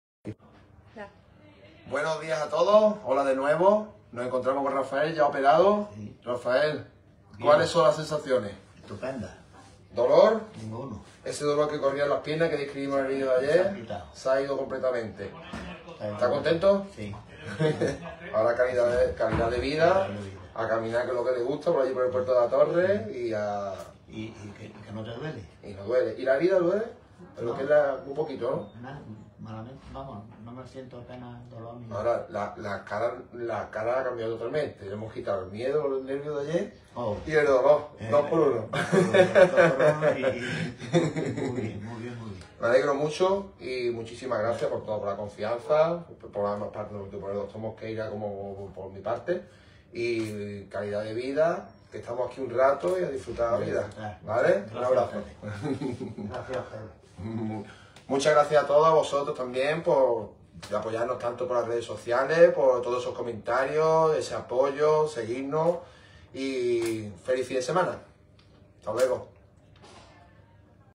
Rafael en menos de 24 horas - Clínica Dr. Mosqueira
¡Así se encuentra Rafael a menos de 24 horas de su intervención con microcirugía minimamente invasiva! Comienza una nueva vida.
☎️ Pide cita en el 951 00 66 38